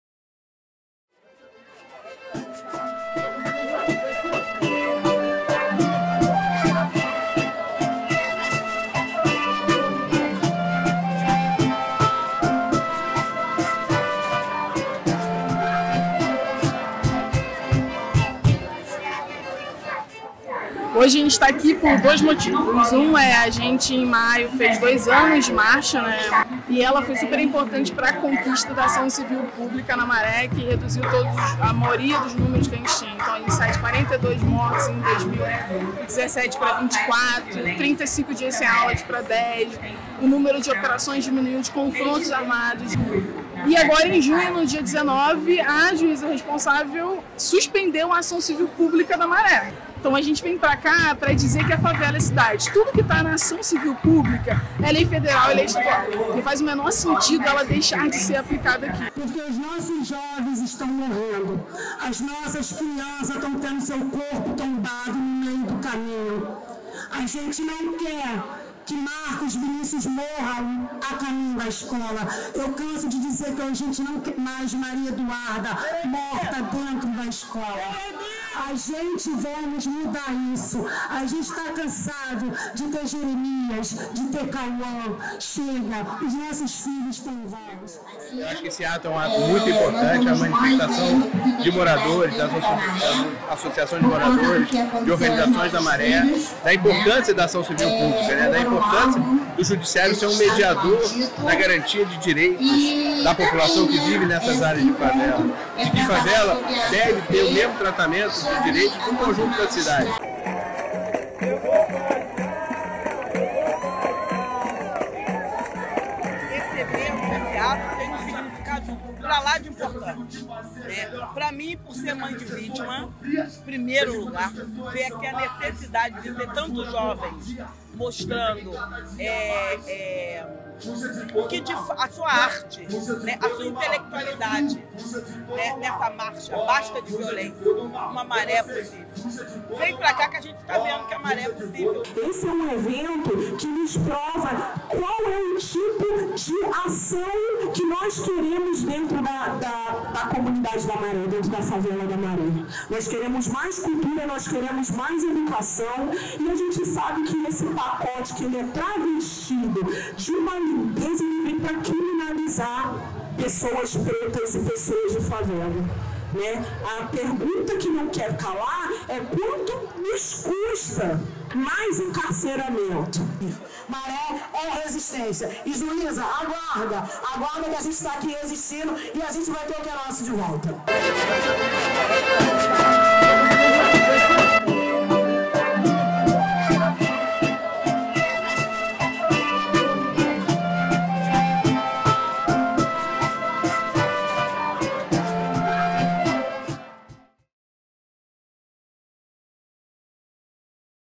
2 anos de Marcha Contra a Violência na Maré
Um ato realizado no dia 10 de julho, na Maré, marcou os dois anos da Marcha Basta de Violência! Outra Maré é Possível, manifestação promovida pelo Fórum Basta de Violência! Outra Maré é Possível, em 2017, em resposta ao cenário de muitas mortes e operações truculentas que aconteceram no período. O ato, realizado em uma região simbólica na Maré, a “Divisa”, um território marcado por confrontos armados frequentes, também teve como objetivo protestar contra a suspensão da Ação Civil Pública da Maré (ACP).
O ato foi marcado por diversas atividades e falas, entre elas: mutirão de revitalização, cineclube, capoeira, a presença da Roda Cultural do Parque União e da Orquestra Maré do Amanhã. Depoimentos de moradores e integrantes de organizações locais sobre o contexto de segurança pública da Maré merecem destaque.